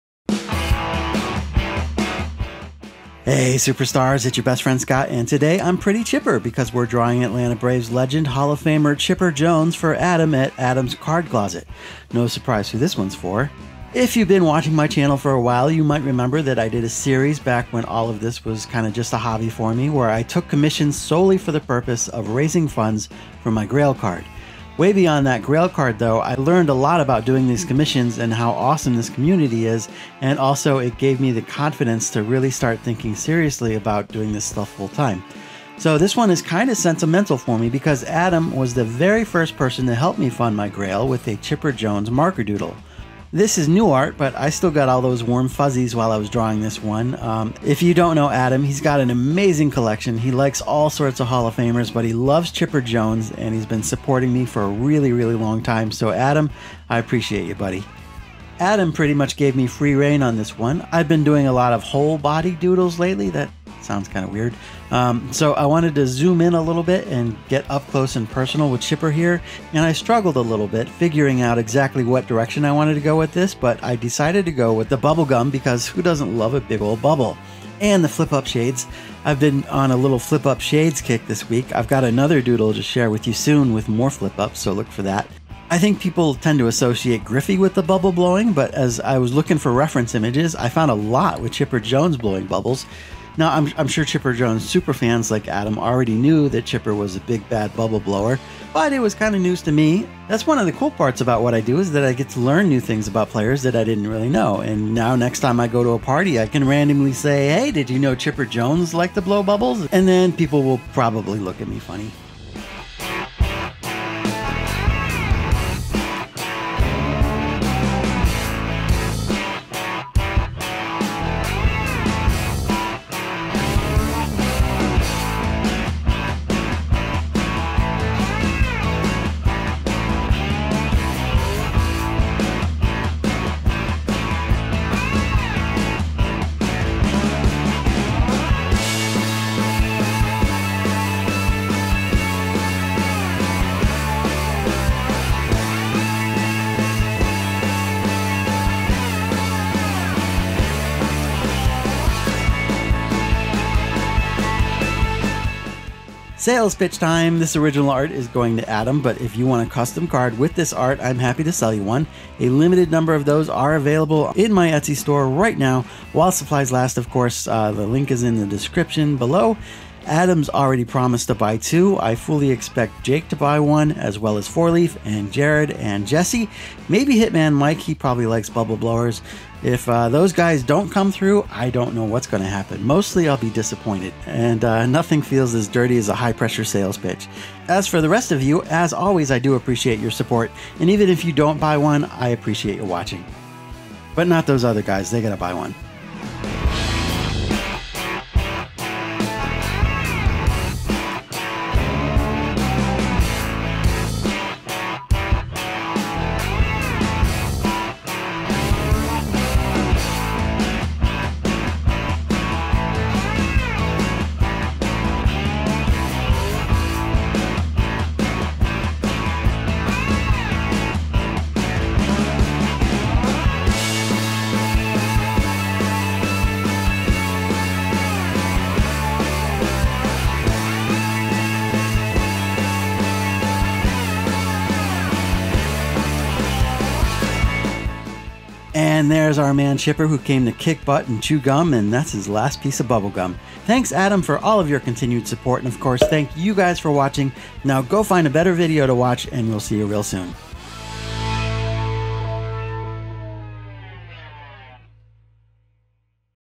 Chipper Jones | Marker Doodle Monday
Today, I’m drawing Atlanta Braves’ Legend and Hall of Famer, Chipper Jones for @adamscardcloset9020

If you’d like to support Reindeer Studios, please consider buying something from my Etsy store